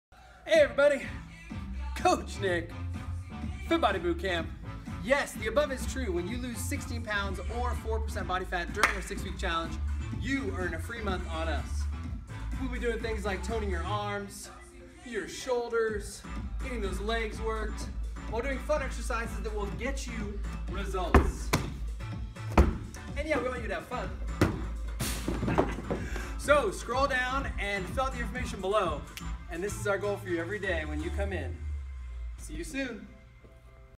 Drop and Earn!
When you drop the pounds during our 6 week challenge, you'll earn a free month!